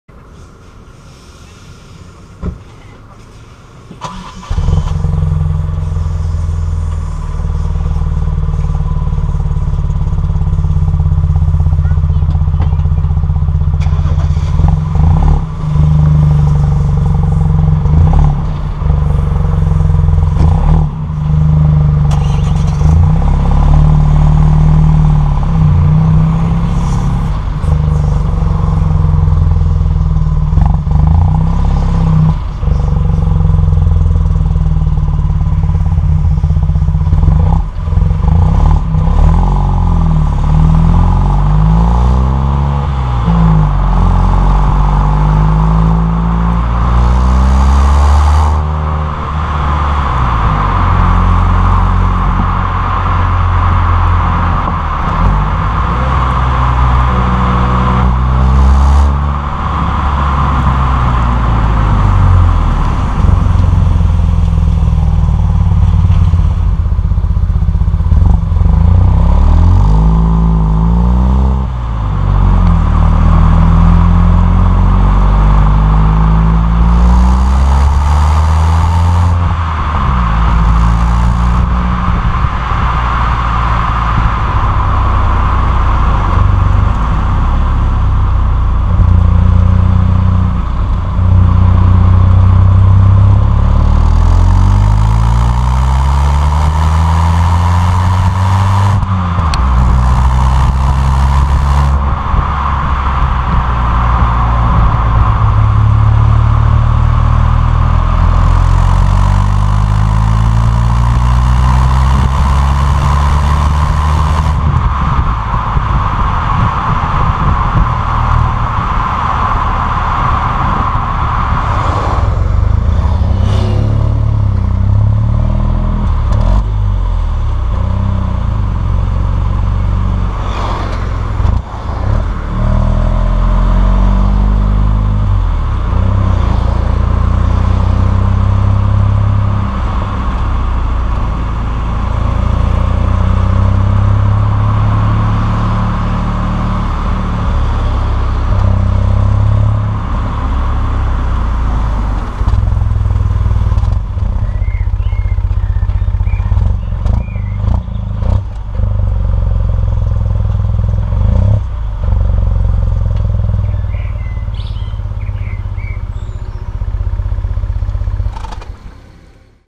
Fiat 500S Twinair 85 || Exhaust Sound GoPro Hero HD3+
Just a little impression of the sound coming from the little 0.9l two-cylinder engine with a stock exhaust. This car has been chipped from 85hp to 103hp. I like the character this car has, it feels funny, compared to the four-cylinder version :)